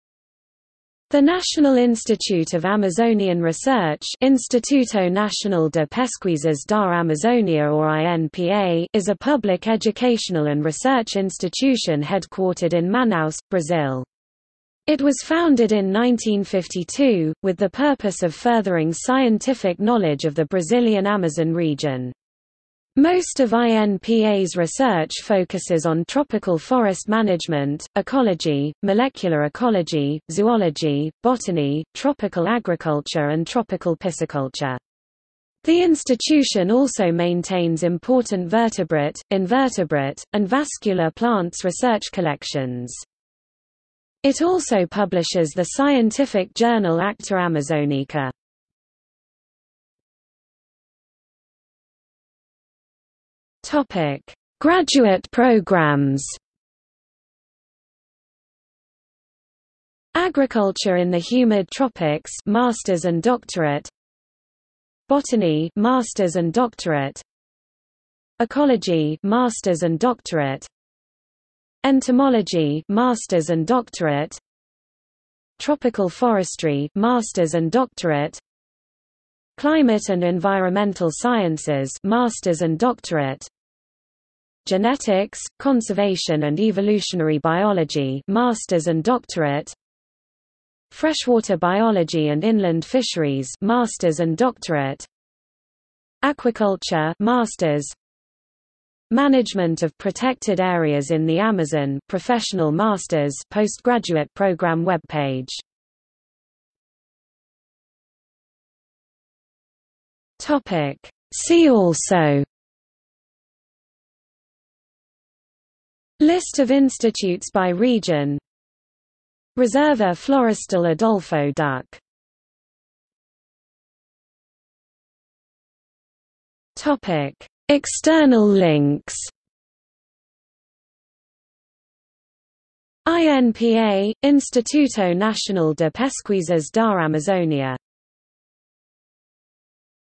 National Institute of Amazonian Research | Wikipedia audio article
00:00:50 Graduate programs
00:01:46 See also
00:02:03 External links

- Socrates

SUMMARY
The National Institute of Amazonian Research (Instituto Nacional de Pesquisas da Amazônia or INPA) is a public educational and research institution headquartered in Manaus, Brazil. It was founded in 1952, with the purpose of furthering scientific knowledge of the Brazilian Amazon Region. Most of INPA's research focuses on tropical forest management, ecology, molecular ecology, zoology, botany, tropical agriculture and tropical pisciculture. The institution also maintains important vertebrate, invertebrate, and vascular plants research collections.
It also publishes the scientific journal Acta Amazônica.